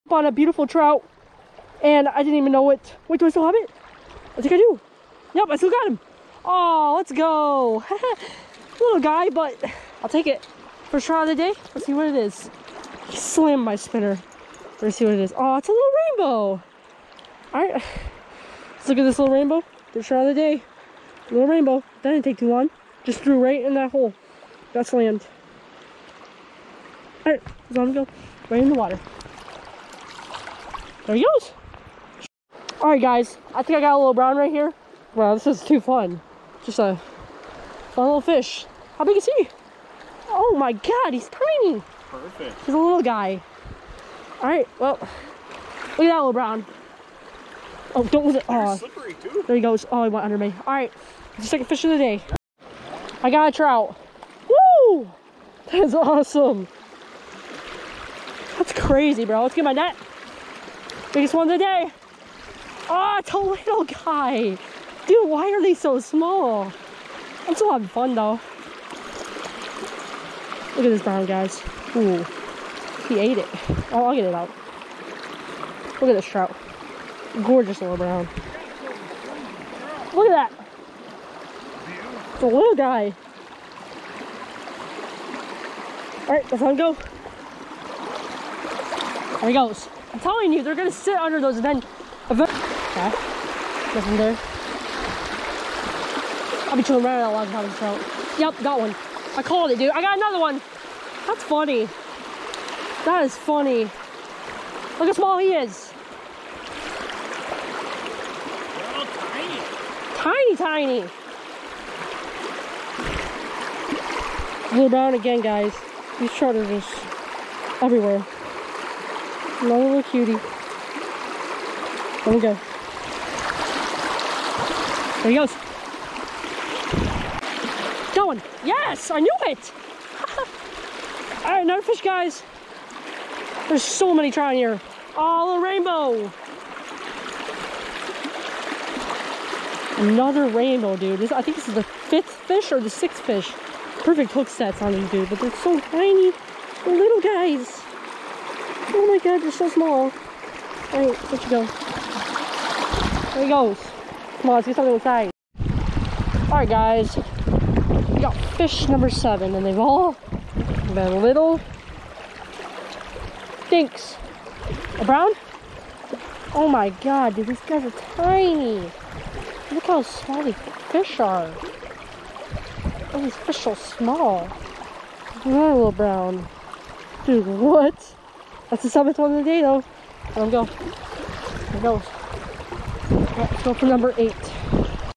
Trout Fishing The Boardman River! 😄
In all its a fast flowing good River but just not many big trout. I was trying to at least get one over 10 but the biggest was like 8 so I mean it's a good River for numbers of trout but not for big ones. Also the vid is in 4K!! Rainbows and Browns.